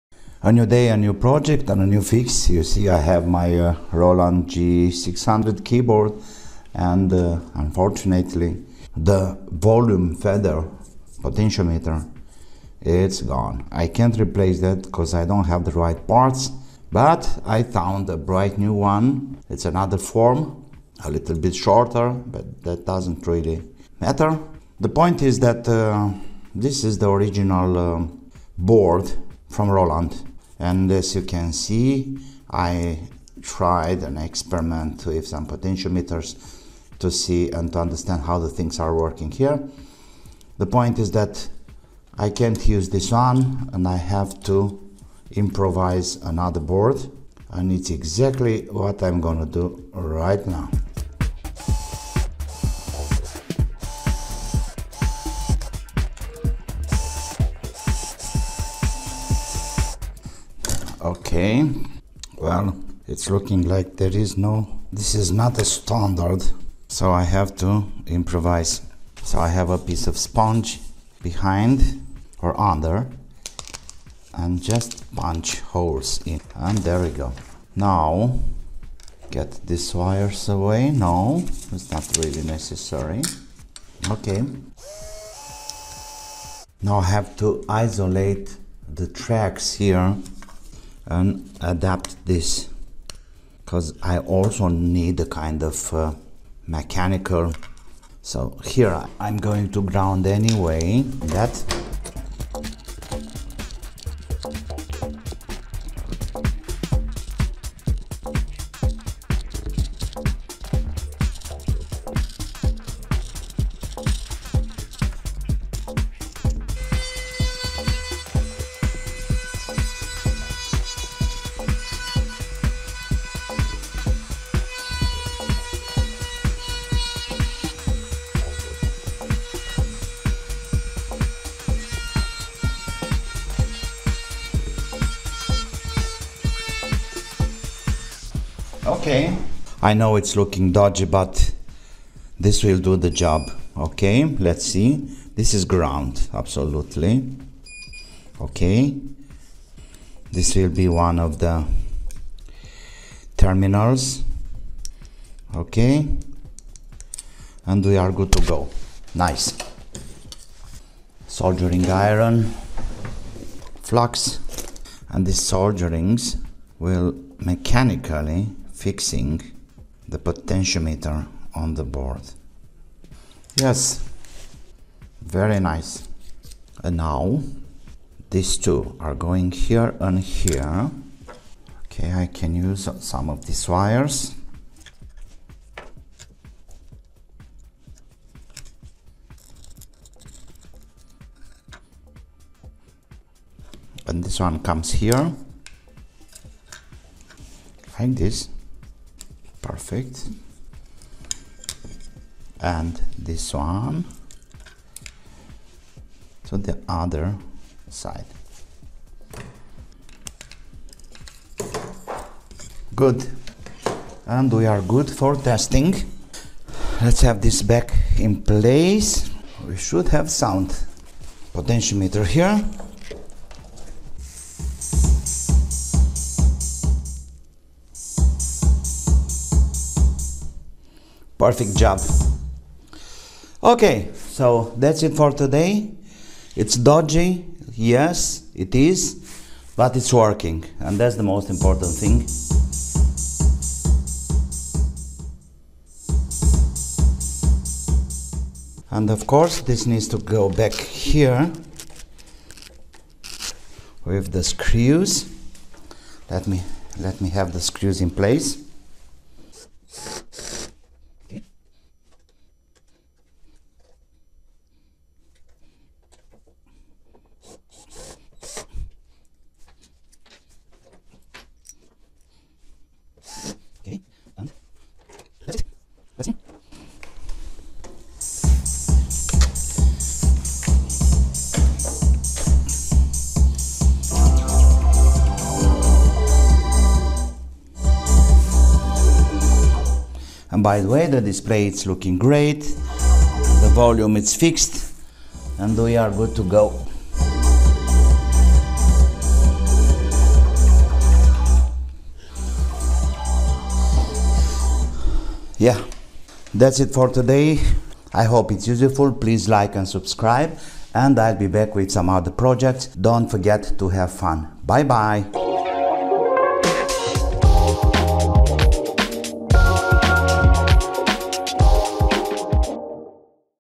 Roland G600 volume potetiometer fix
Improvising a new volume fader potentimeter Roland G600..and many other models uses  linear slide faders.